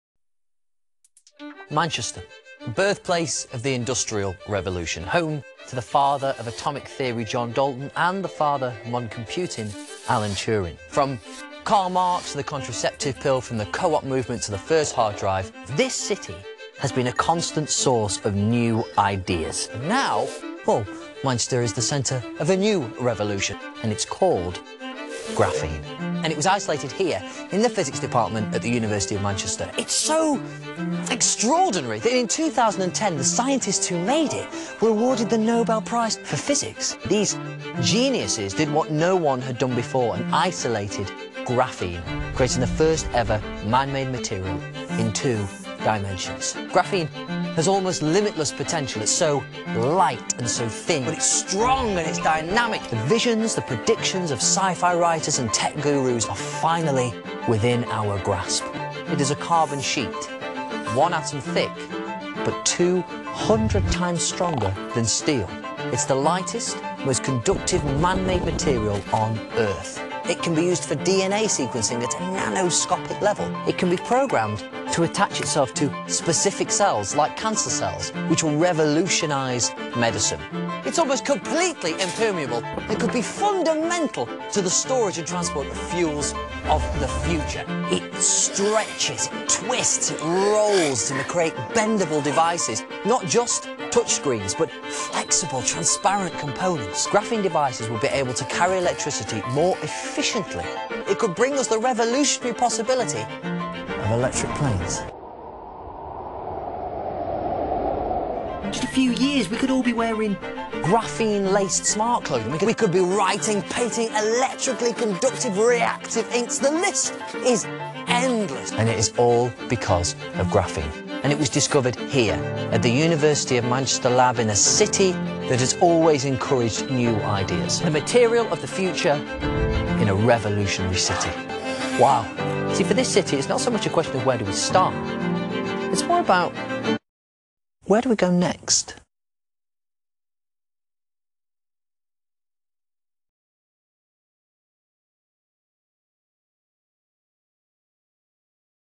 💎Discovery of graphene💎
Fascination with this material stems from its remarkable physical properties and the potential applications these properties offer for the future. Although scientists knew one atom thick, two-dimensional crystal graphene existed, no-one had worked out how to extract it from graphite.
That was until it was isolated in 2004 by two researchers at The University of Manchester, Professor Andre Geim and Professor Kostya Novoselov. This is the story of how that stunning scientific feat came about and why Andre and Kostya won the Nobel Prize in Physics for their pioneering work.